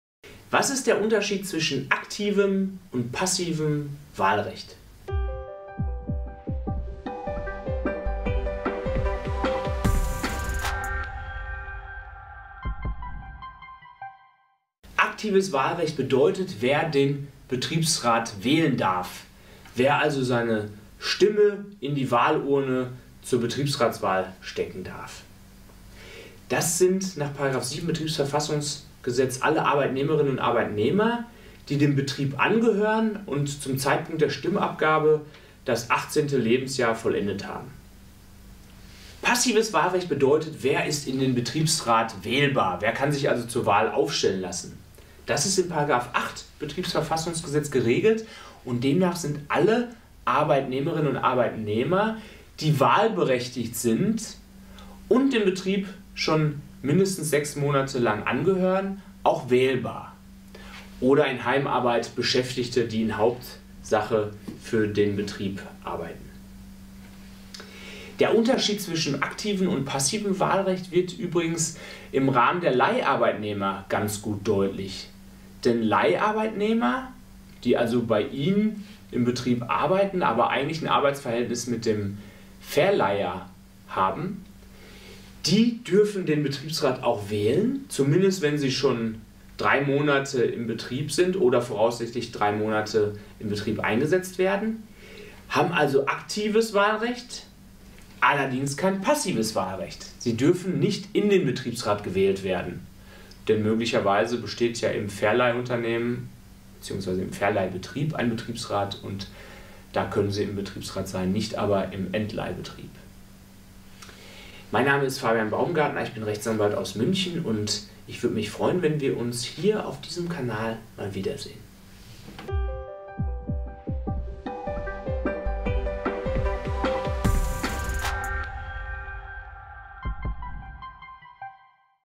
Aktives und passives Wahlrecht bei der Betriebsratswahl - Wo ist der Unterschied?
Unser W.A.F. Referent zeigt ihnen den Unterschied zwischen aktiven und passiven Wahlrecht bei der Betriebswahl.

🕘 Inhalt:
0:00 - Intro
0.45 - Wer kann sich bei der Wahl aufstellen lassen?
1:10 - Aktives und passives Wahlrecht

SOCIAL MEDIA

WISSEN & AUSTAUSCH